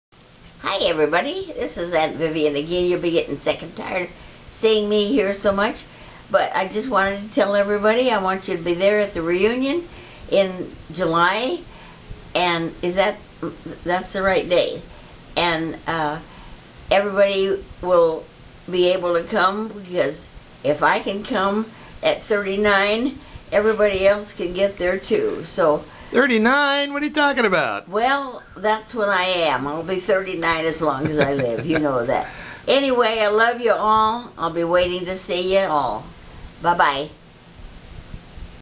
Vivian's Reunion Reminder.wmv
Our Precious Aunt Vivian reminds us all about the upcoming Daley Family Reunion in July of this year.  As she says if she can come at 39(93) then there are no excuses for any not to make it to Oceanside this Summer.
Love the Apple/Cherry Slurpee she is enjoying.  Make plans to see th oldest living legend in our family when you can...